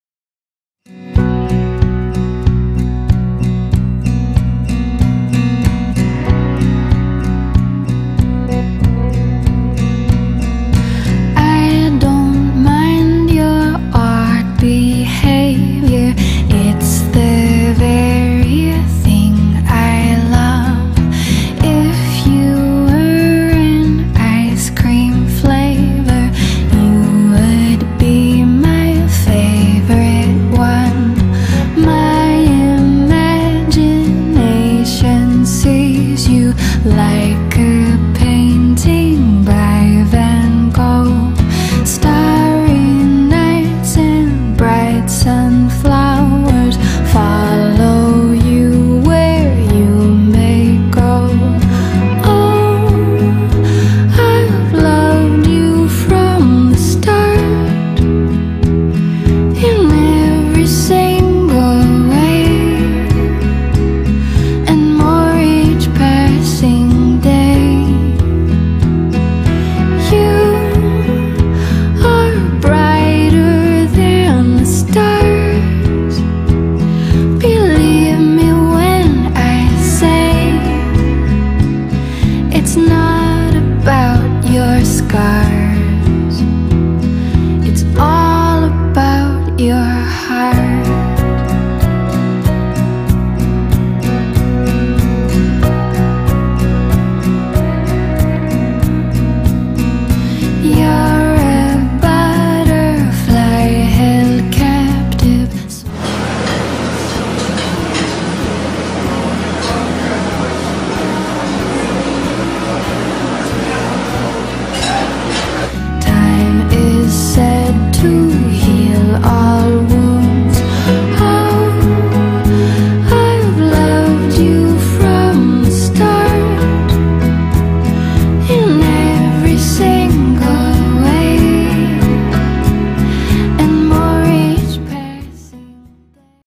Vancouver Commercial Wine Cellar Design - Salam Bombay Indian Restaurant Project
This contemporary wine display cellar was custom designed and built for the Salam Bombay Indian Restaurant.
The watch the process of transforming a standard modular wine rack display into an impressive refrigerated and back lit wine cellar.  With this project it's easy to see why wine sales are so positively impacted with a quality wine presentation.
The Salam Bombay restaurant is situated at Suite # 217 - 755 Burrard Street, Vancouver, British Columbia, Canada